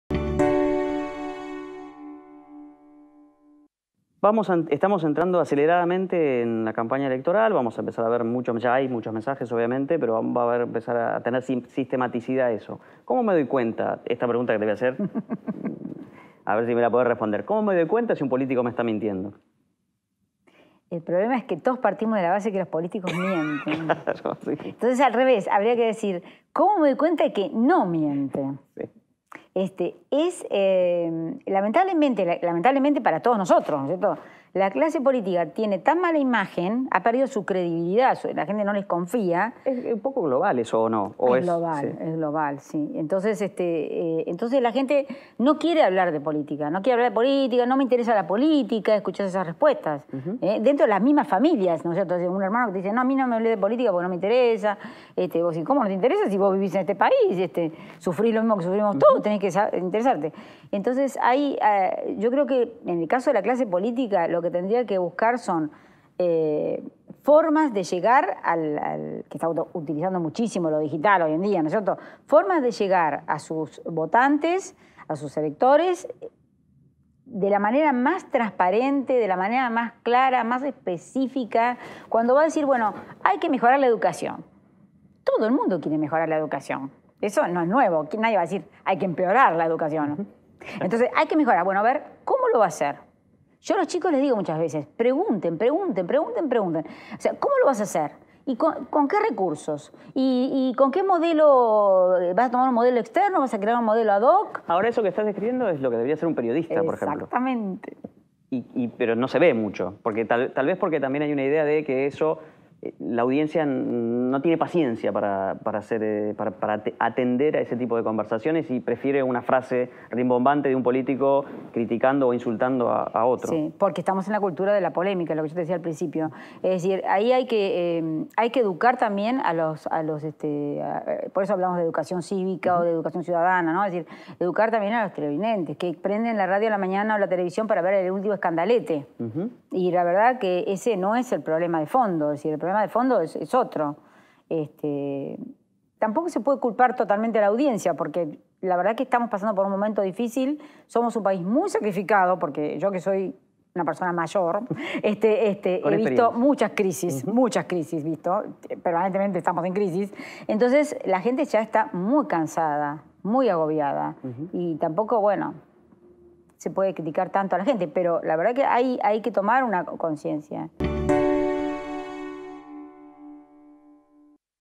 ¿Cómo me doy cuenta si un político está mintiendo? - Dra. Patricia Nigro
Entrevista a Patricia Nigro

TMT Conversaciones con José Crettaz